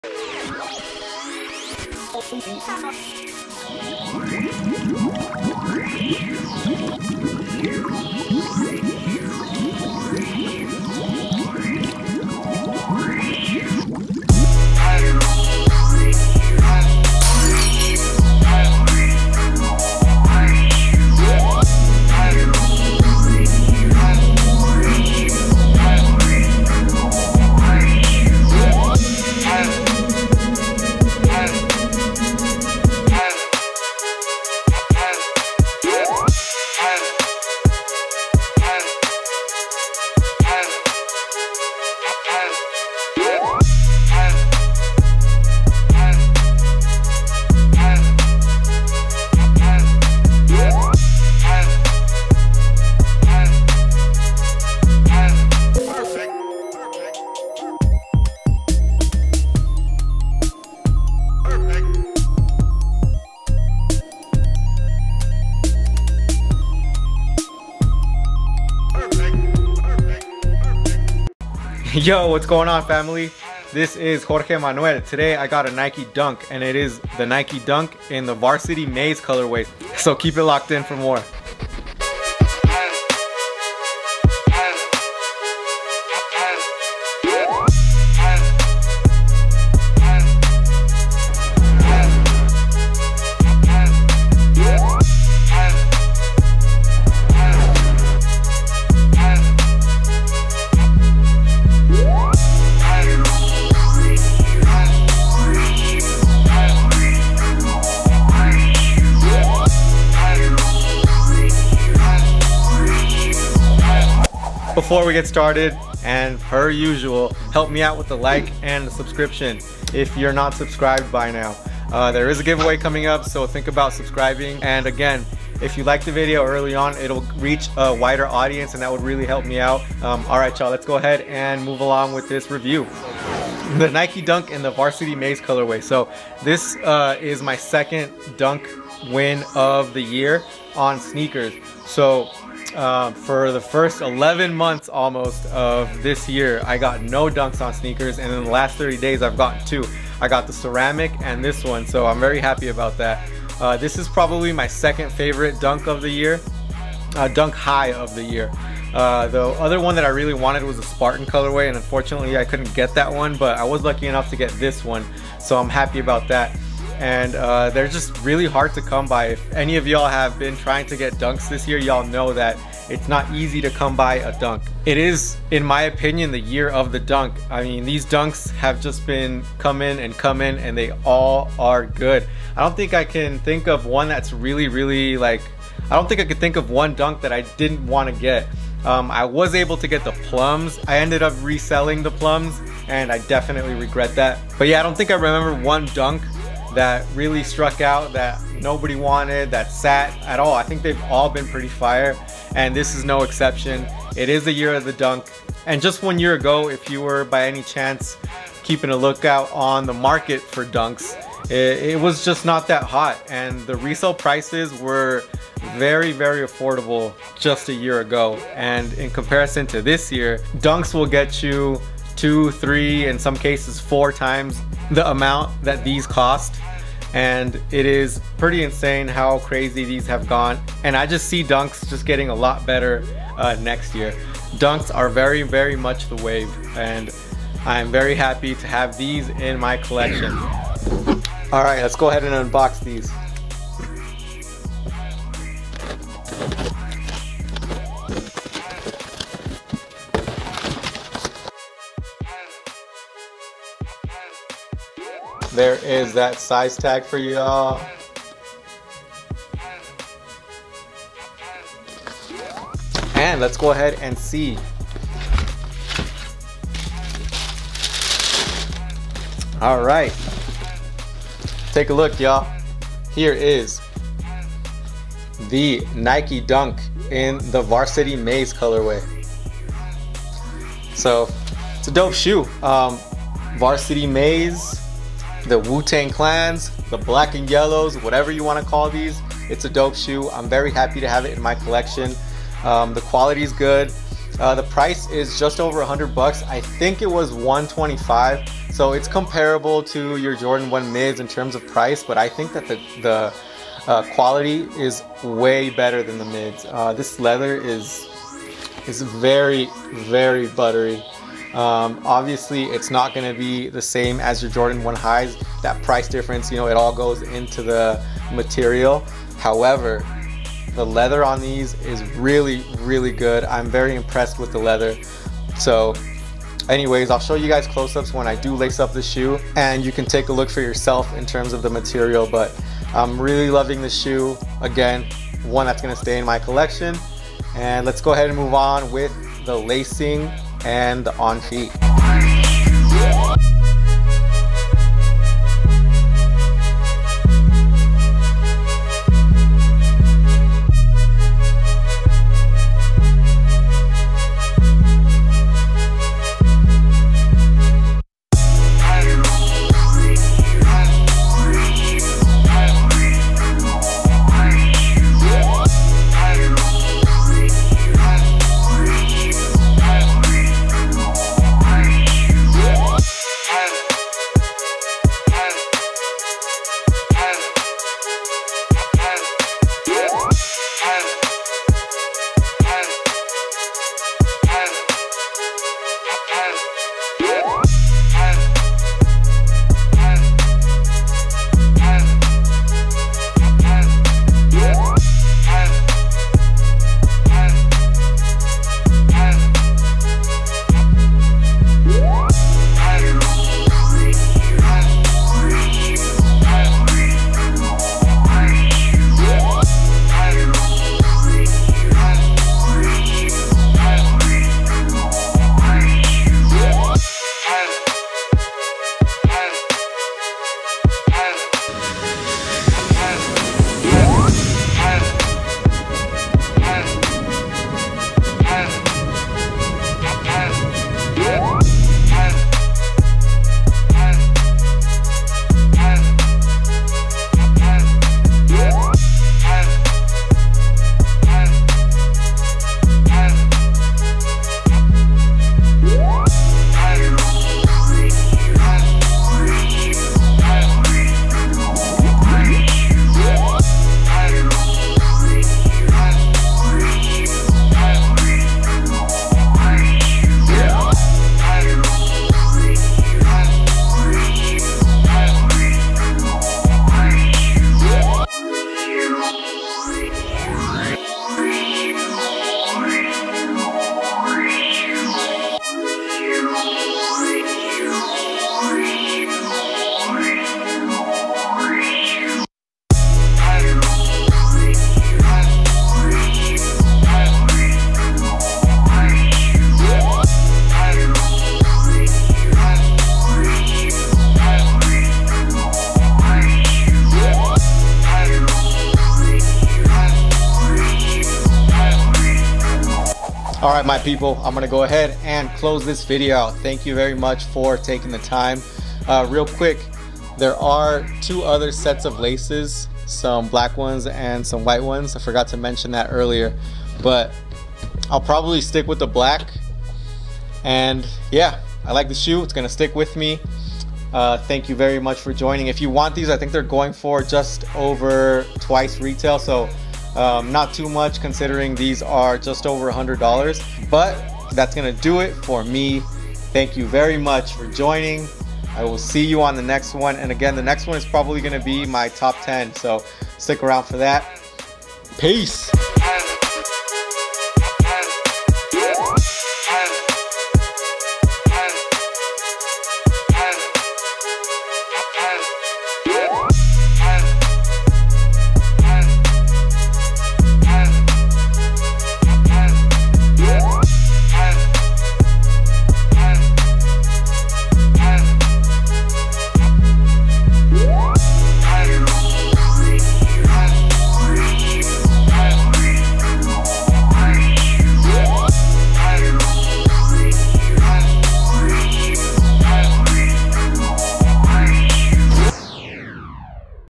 Nike Dunk High Varsity Maize / Iowa / Wu Tang Clan - Review & On Foot!
Just a quick video on the Nike Dunk High Varsity Maize / Iowa.  These closely resemble the very limited wu tang clan dunks and were a must for me.  Hope y'all enjoy the closeups and on feet on this sneaker review.  The leather is outstanding considering the price on these.

Next two videos will be my top 10 of the year and then I'll take a little two-week holiday break.  I'll catch y'all on the next vid!

J